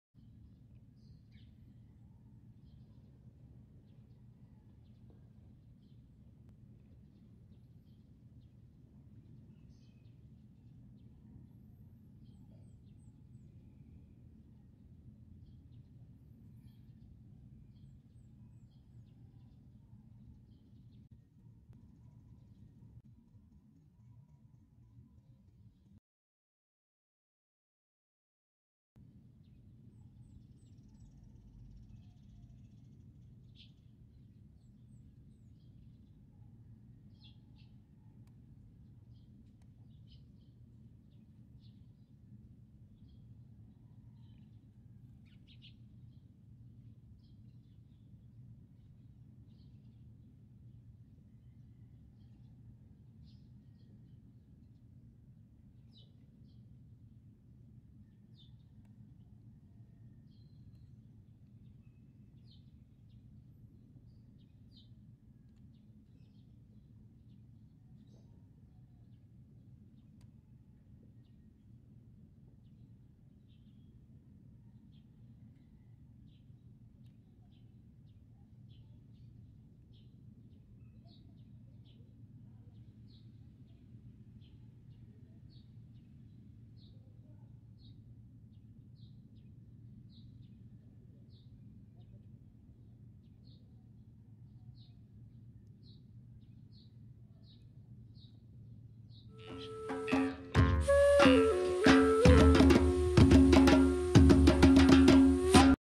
Fiddle crab (agokoy in Cebuano) digging its home on white sand.
When you spend time alone in one of our many beautiful white sand beaches, I'm more than certain you will get to see these "holes".

In case you're wondering what made them, these guys did!

At least for this particular one as some beaches might have other digging creatures as well like the mud lobster or "mangla" in bisaya.